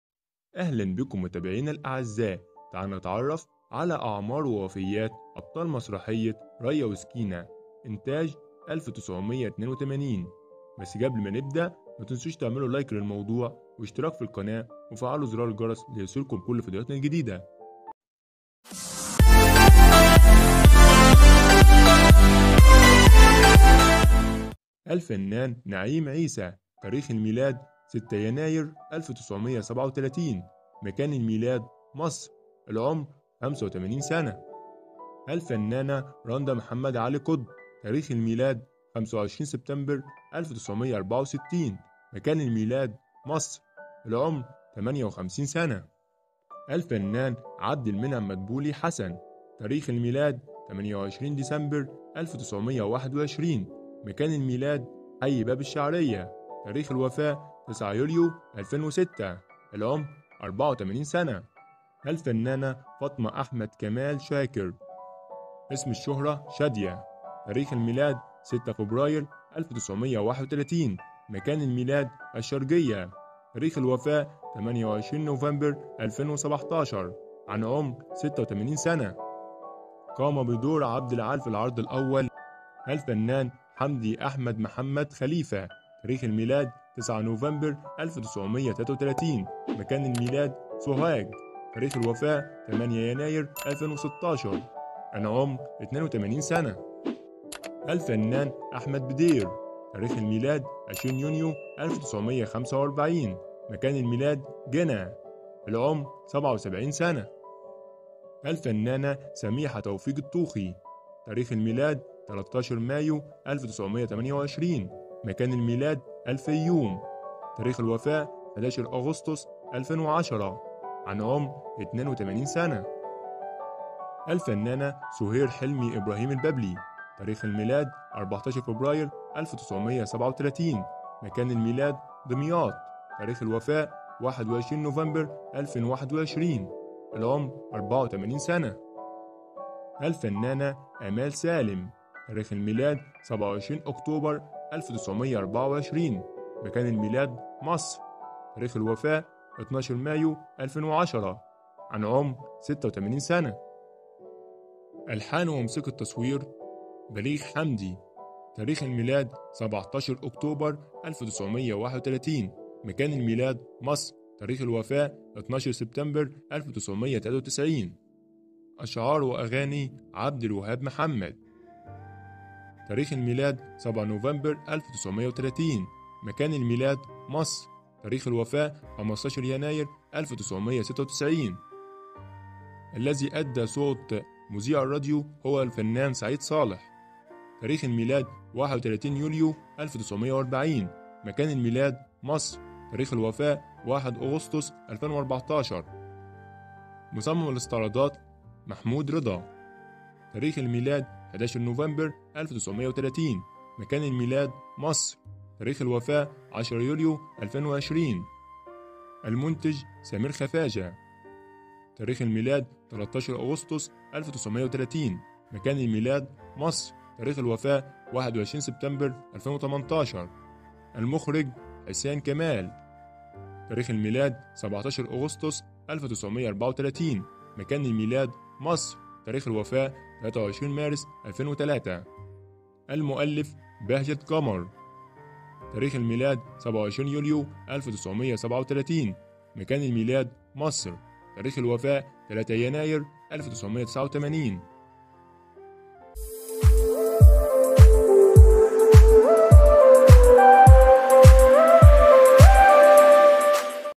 اعمار وفيات ابطال مسرحيه ريا وسكينه وكل القائمين بهذا العمل
في هذا الفيديو سوف نتعرف على ابطال مسرحيه ريا وسكينه انتاج 1982
 الفنان احمد بدير
 الفنان ابراهيم مدبولي
 الفنانه شاديه
 الفنانه سهير البابلي
 الفنانه امال سالم
 الفنانه سميحه توفيق الطوخي
 الفنان حمدي احمد محمد
 الفنانه راندا محمد علي قطب
 الفنان نعيم عيسى